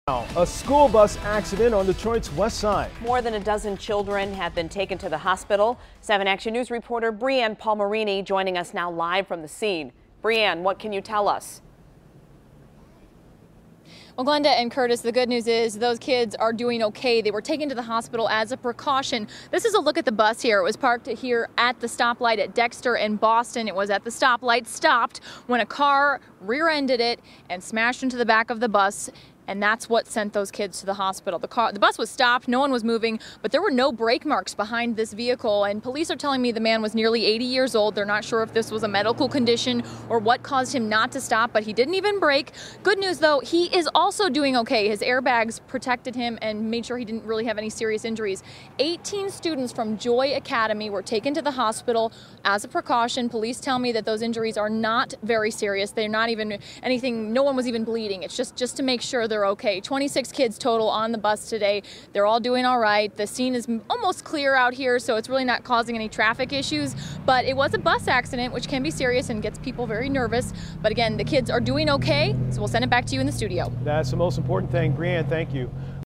18 kids hospitalized after school bus accident in Detroit
18 kids hospitalized after school bus accident in Detroit